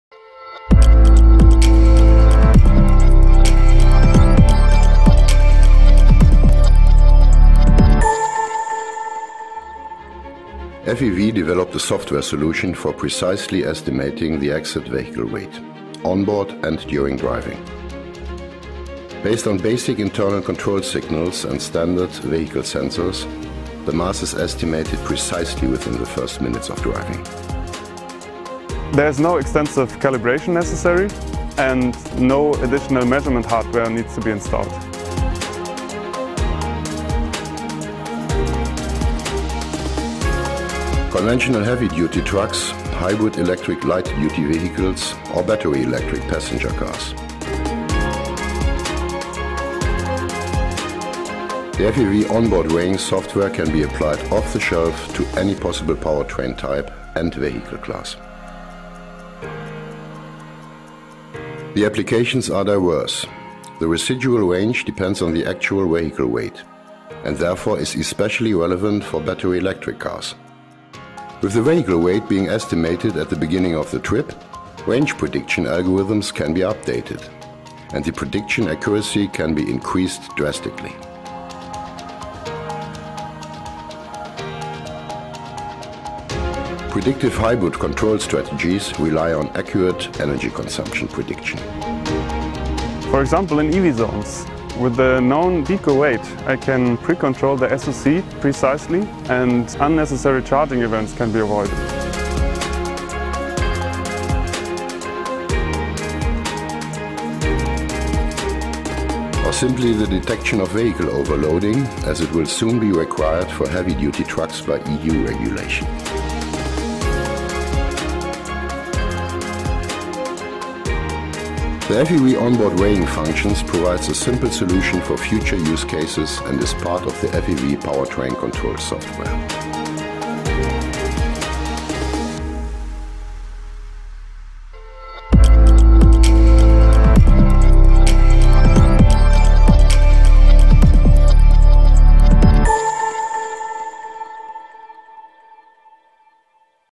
FEV On Board Weighing Function
With new on board weight regulations for commerical vehicles coming in 2021 in the EU, FEV’s software solution for real time vehicle weight determination can be integrated into any (EV) vehicle type.